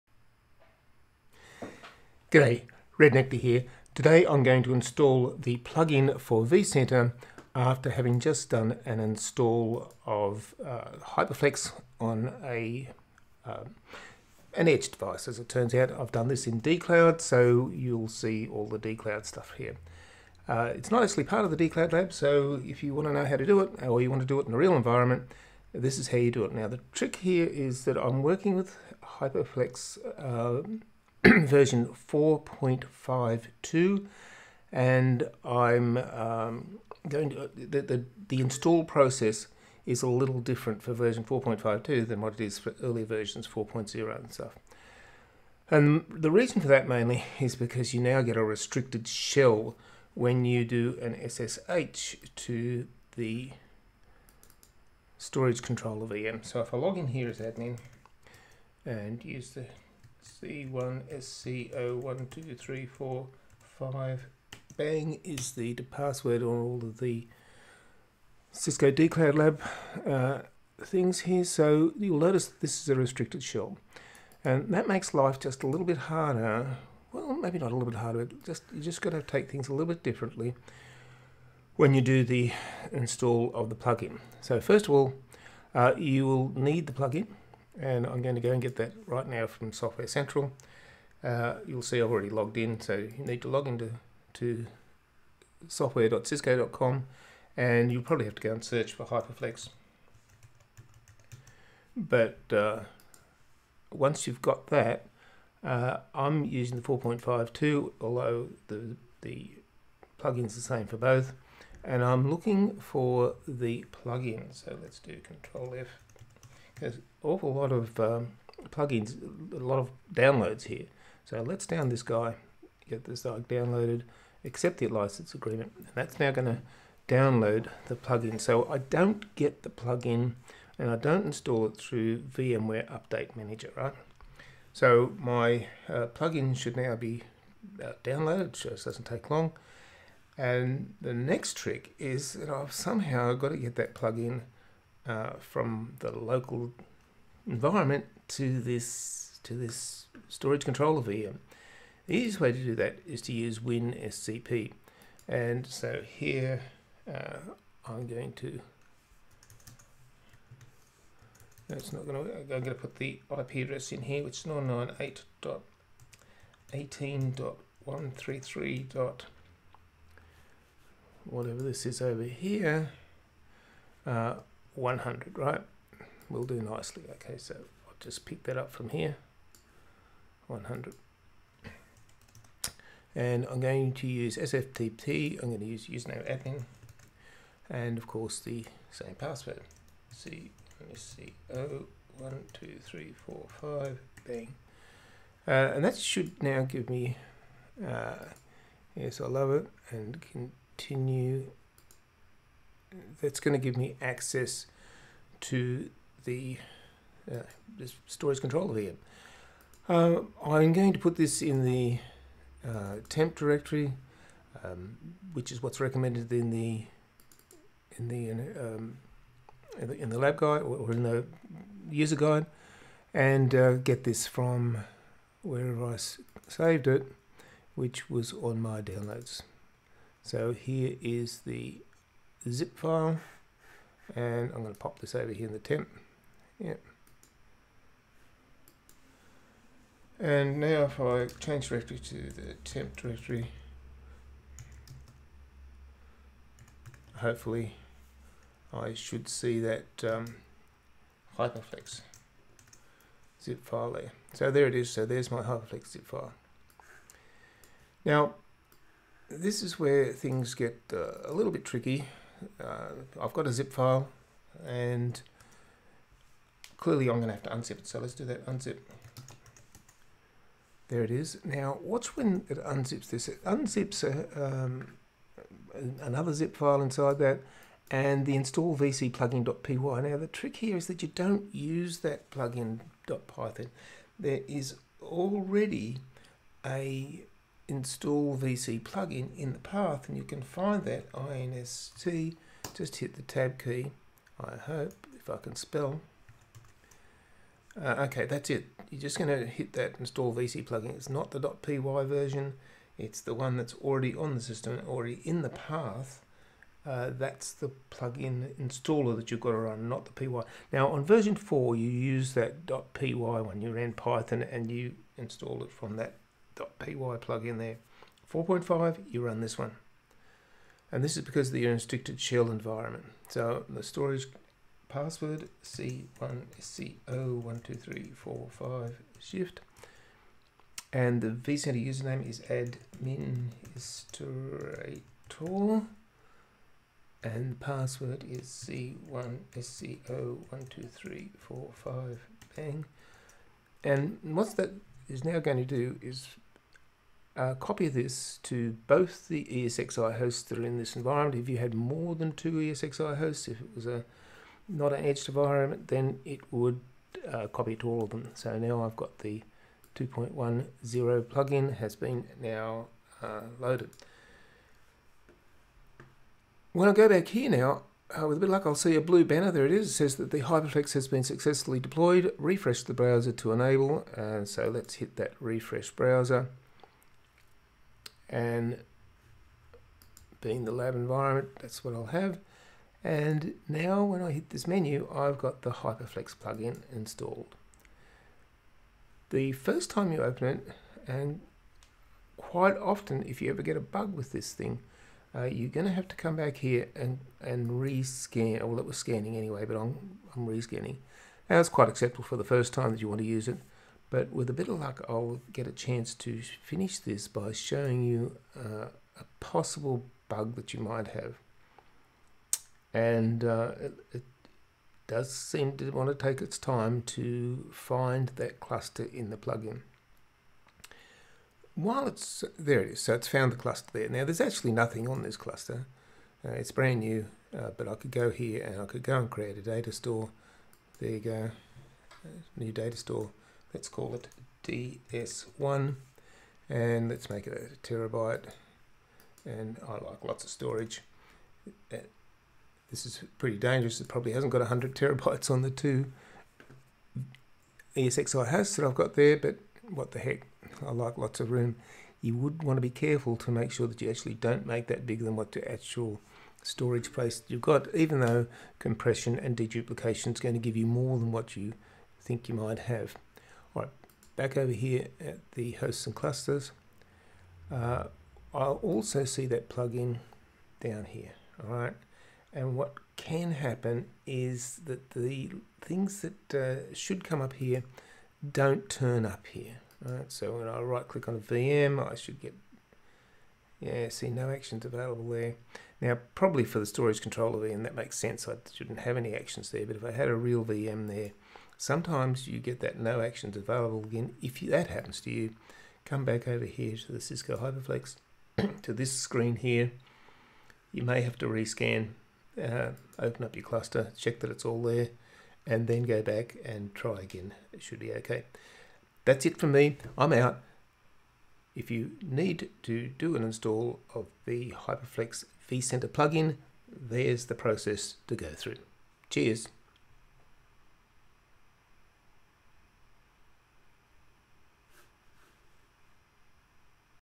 Installing HyperFlex plugin on vCenter is different now
Installing the vCenter plugin for Cisco's HyperFlex Data Platform changed in v4.5 - here's how its done now